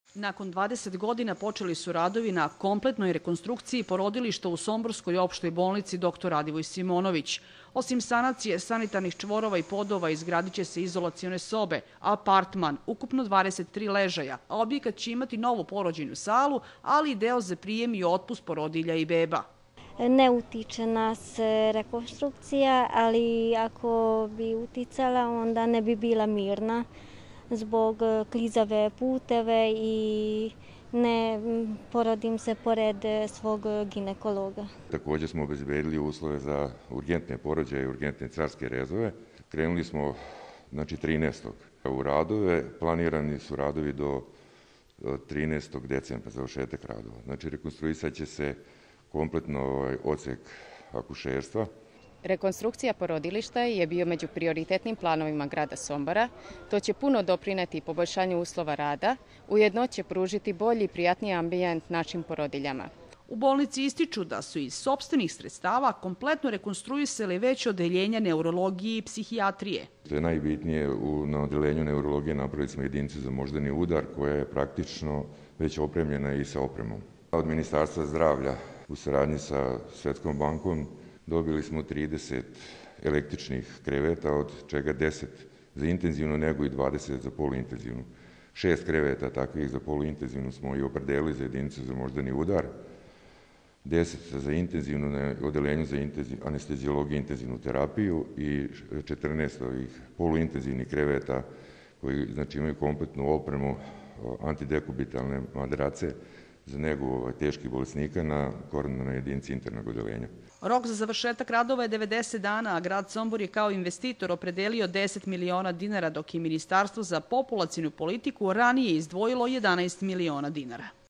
Zbog rekonstrukcije somborskog porodilišta trudnice upućene na Suboticu i Novi Sad
Zbog rekonstrukcije porodilišta u somborskoj Opštoj bolnici sve pacijentkinje, odn. trudnice će se do završetka radova upućivati u Opštu bolnicu Subotica ili na Kliniku za ginekologiju i akušerstvo Kliničkog centra Vojvodine. I ako su trudnice upućene u druge zdravstvene ustanove, za hitne slučajeve su obezbeđeni uslovi za porođaj u sklopu Odeljenja za ginekologiju. Ukupna vrednost investicije je 21 milion dinara.